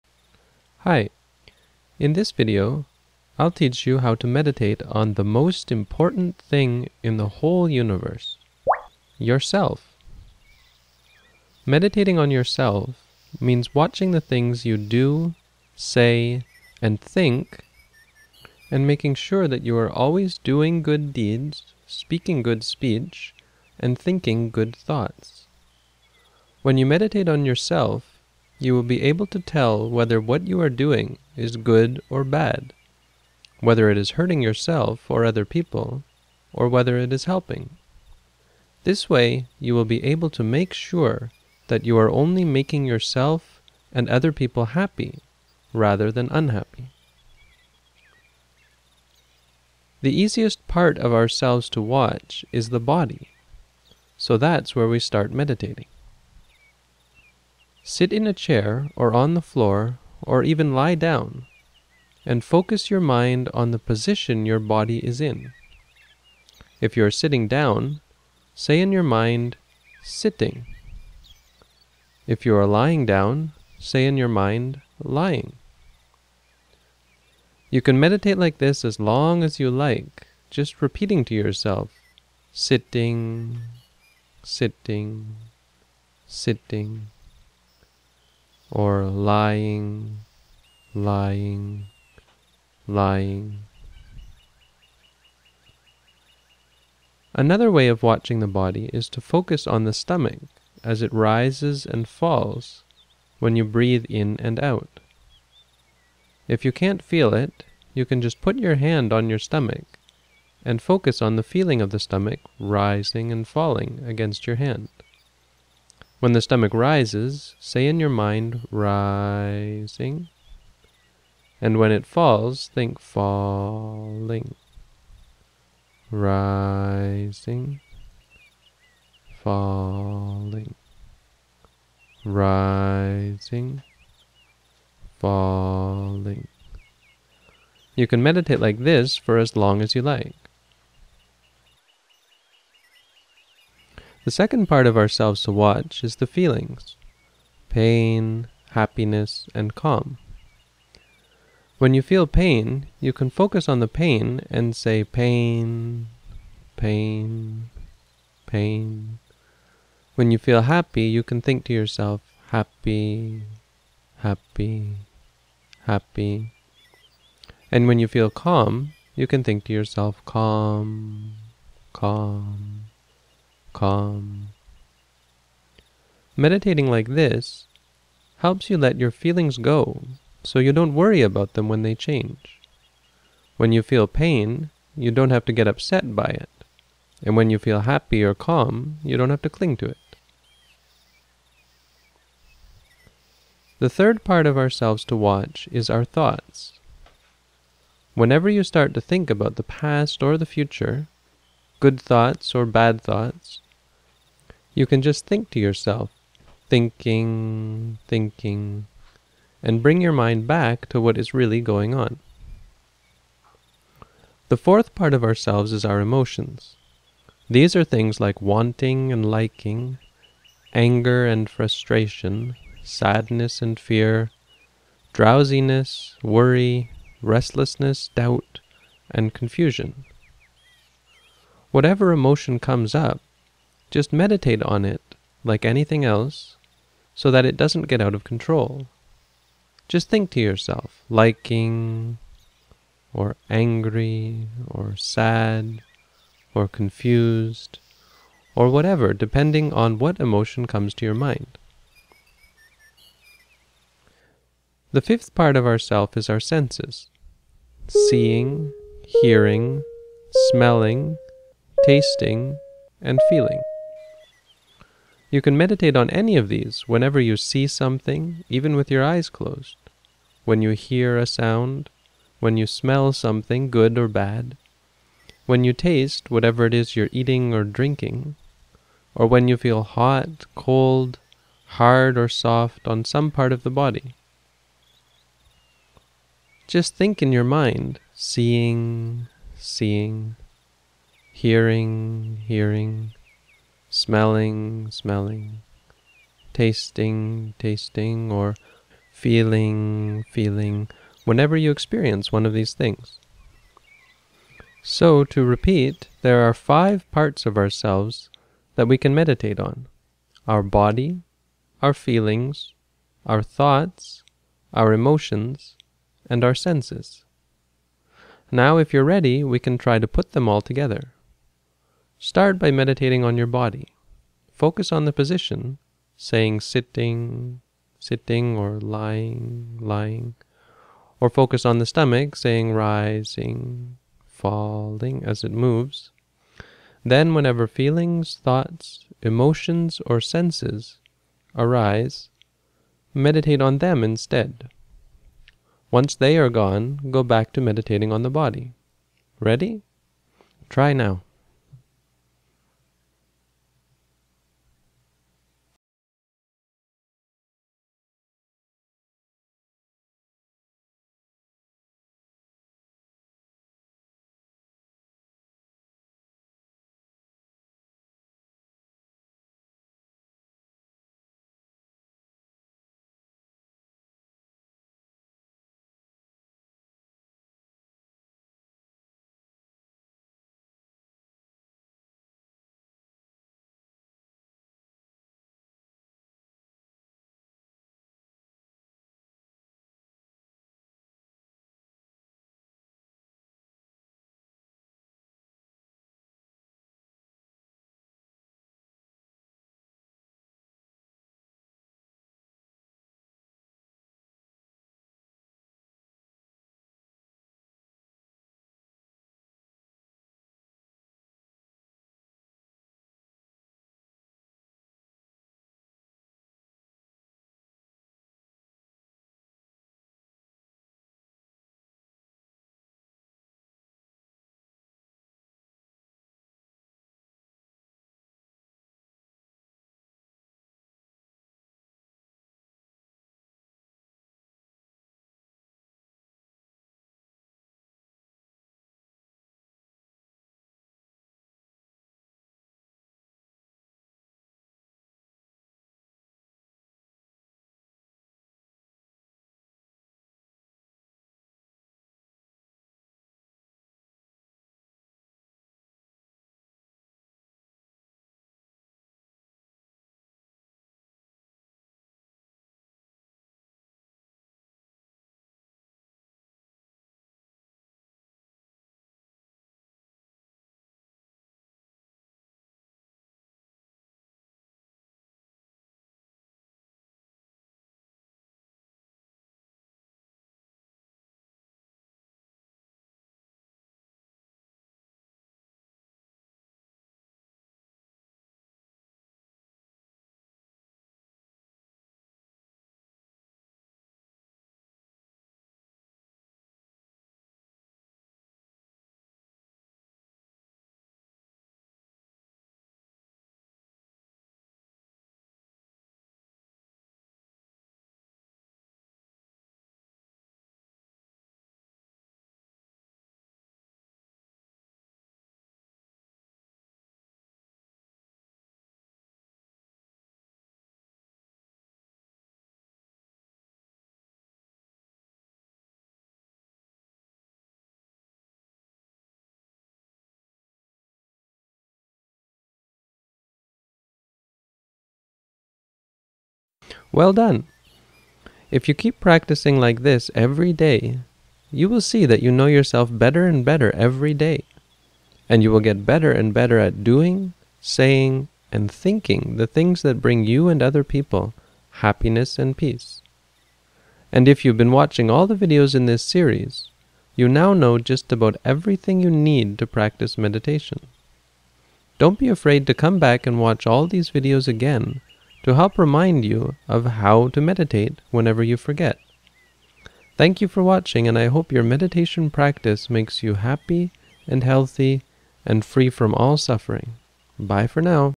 How To Meditate For Children #4: Yourself
Final video in a series devoted to teaching children how to meditate.  This video teaches insight meditation based on the four foundations of mindfulness (in this video, the fourth foundation is split into two parts).

For more information on this type of meditation, please read my booklet on how to meditate:

and don't forget to click the 'like' button to help promote these videos!  Thanks for your questions, comments and support for what I do.